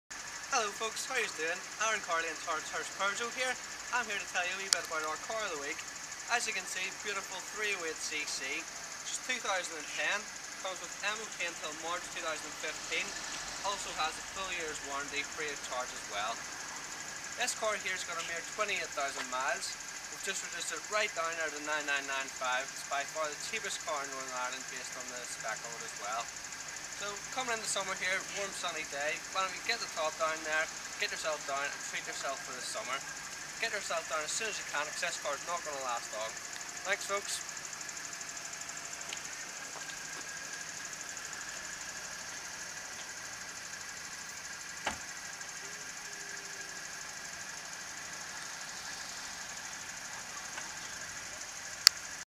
Best Value in NI 308cc
Aaron Carley tells us about this week's car of the week. A Stunning 308cc on a great summers day, no better time to treat yourself. It's bargin at only £9995!